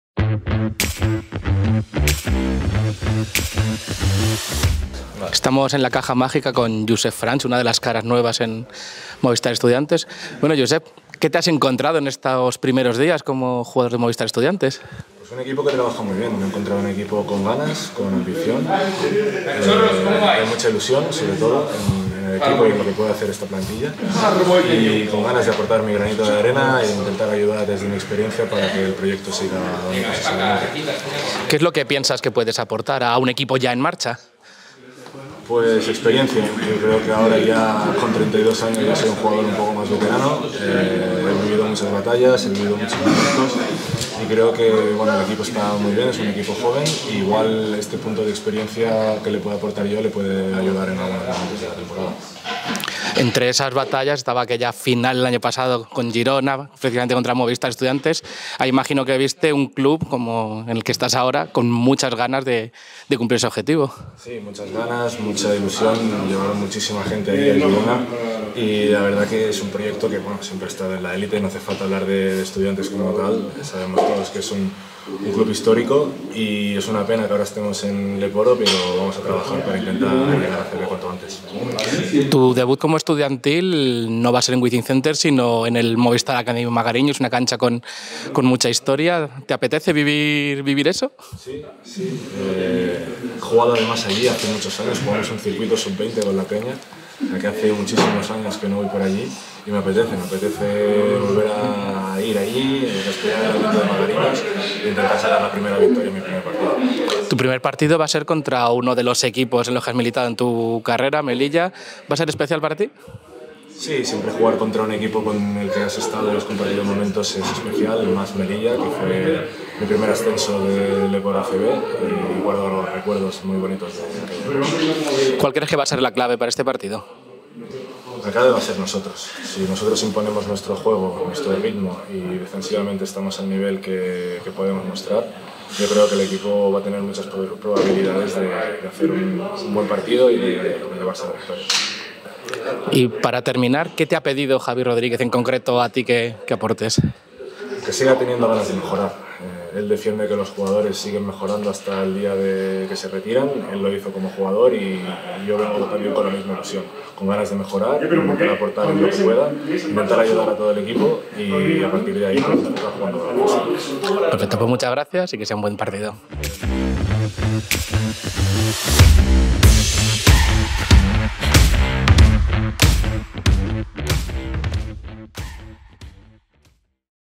Josep Franch:"Mi experiencia puede ayudar en algún momento de la temporada" | MOVISTAR ESTUDIANTES
Josep Franch debutará el viernes en Magariños ante Melilla Sport Capital, equipo con el que consiguió un ascenso a ACB. Franch ha encontrado un equipo con ilusión y calidad en el que su experiencia, con una plantilla tan joven, puede ser importante en lo que queda de temporada.